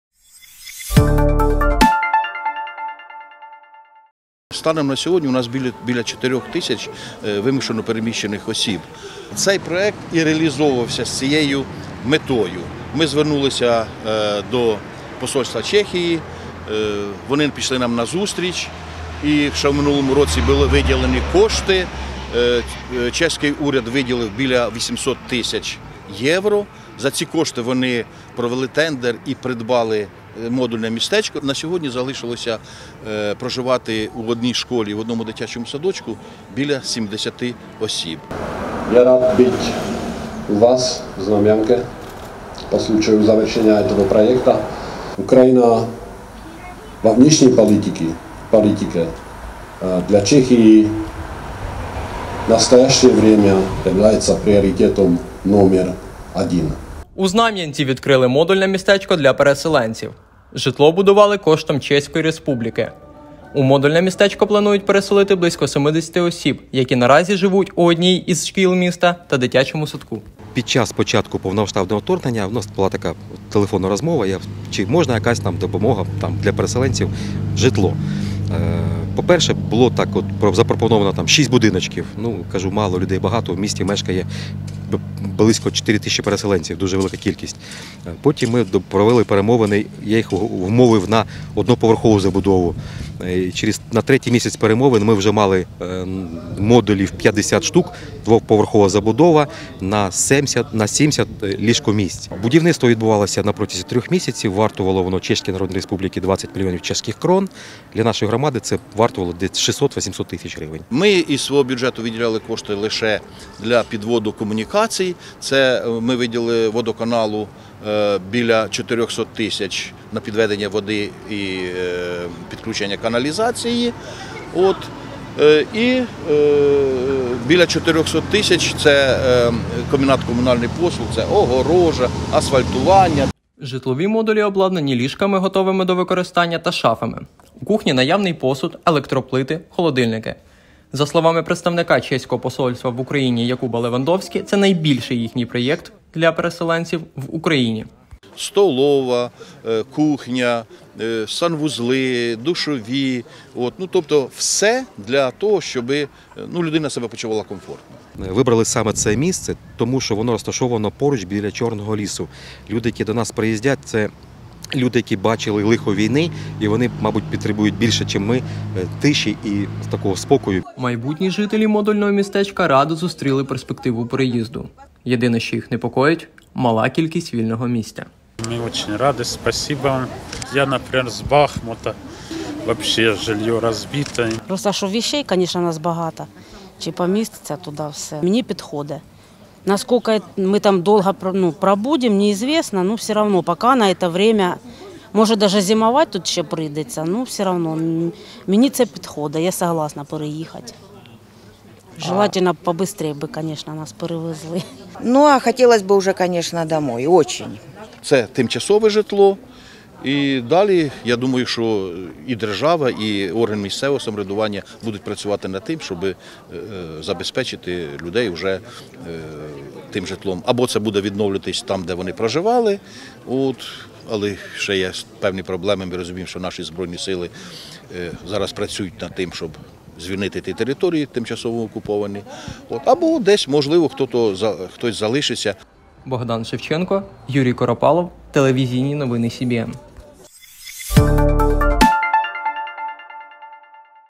Від чехів українцям: у Знам’янці відкрили модульне містечко для переселенців | CBN
У Знам’янці відкрили модульне містечко для переселенців. Житло будували коштом Чеської Республіки. У модульне містечко планують заселити близько 70 осіб, які наразі живуть в одній зі шкіл міста та дитячому садку. Житлові модулі уже мебльовані. У кухні наявний посуд, електроплити, холодильники.